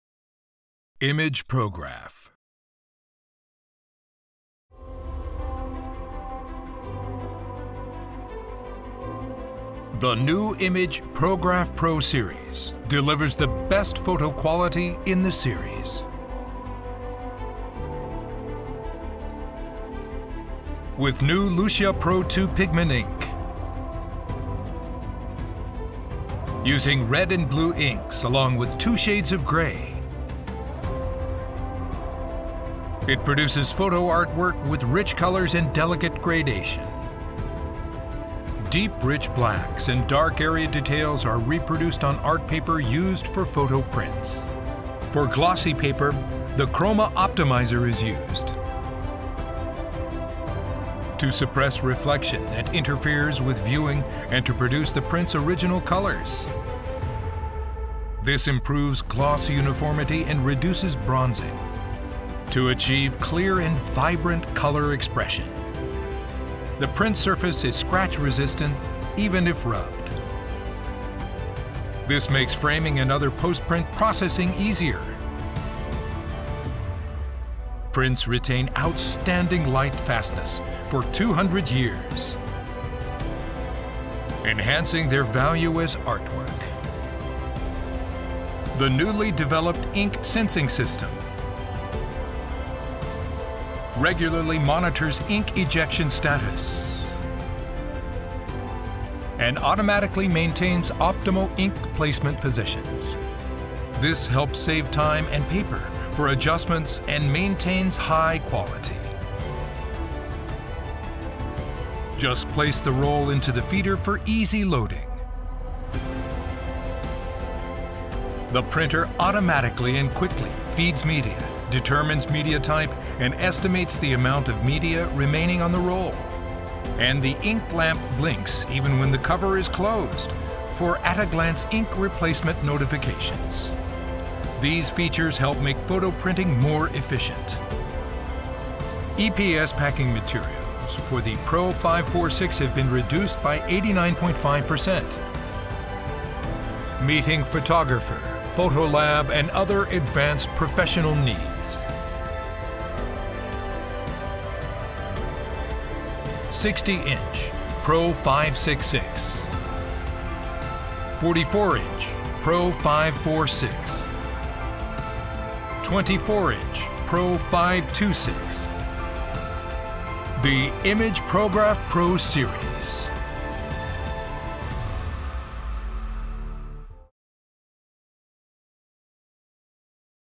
The new imagePROGRAF PRO Series (PRO-526/546/566)
Timeless Quality for Every Print

Stunning images using new 12-colour pigment ink with improved black density is displayed on every print. Greater print expressions of colour that professional photographers can desire.